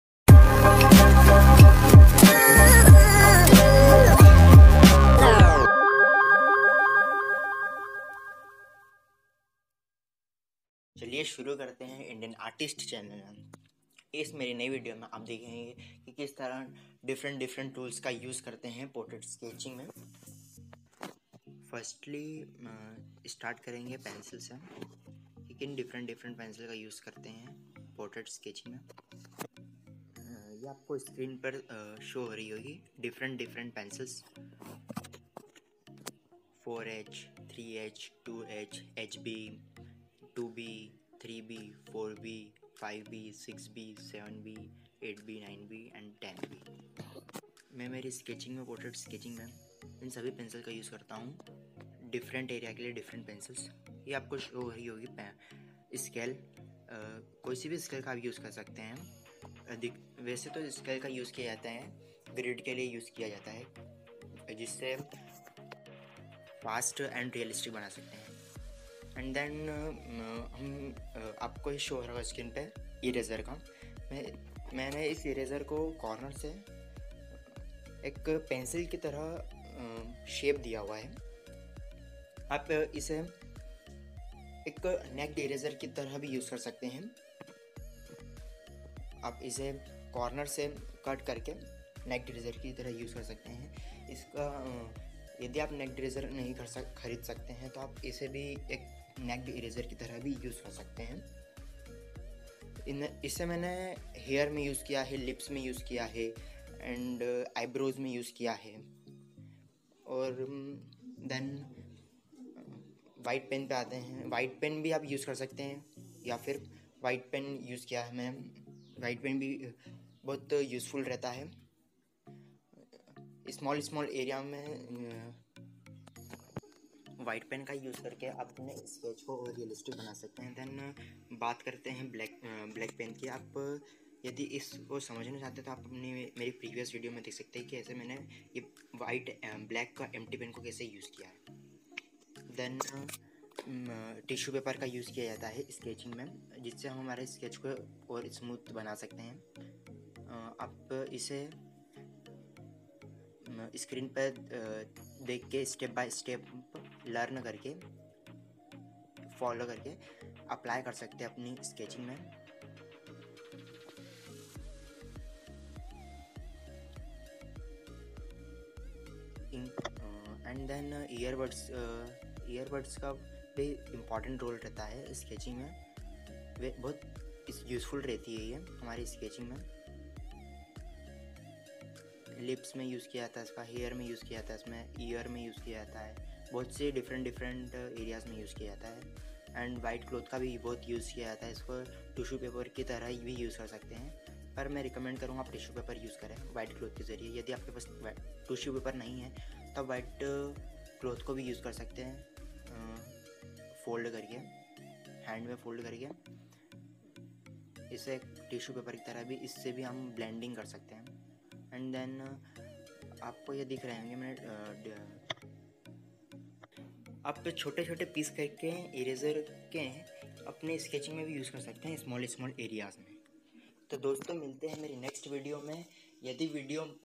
What are the different tools i use in my sketchs Learn for ||kids or Bignners
Published on :-  13July2020

In this video you can learn what types or different tools I use in my sketchs or portrait .This video is specialy for kids or Bignners learn or apply in your Sketchs , drawing and portrait and if you difficulty how to use then drop your comments then i can make another video for you that's helpful for your  art drawing or sketching give your love or support 🙏😍❣️